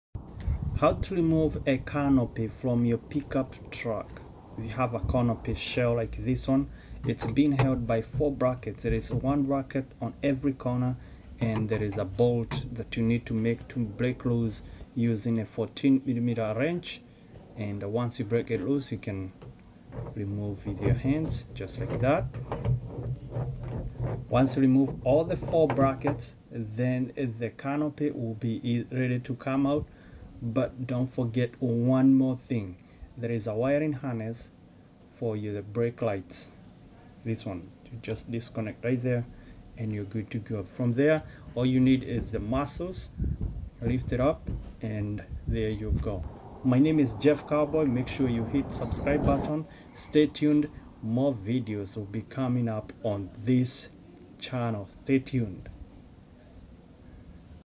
Removing A Camper Shell From Your Pick Up Truck
How To Remove A Camper Shell From Your Pick Up Truck
Some call it camper shell, canopy, pickup truck topper, whatever the name, they are all held by bracket like this one and this was on a 2009 Chevy Corolado Pick-up Truck.

😀Follow Jeff Carbuoy on social media at 😀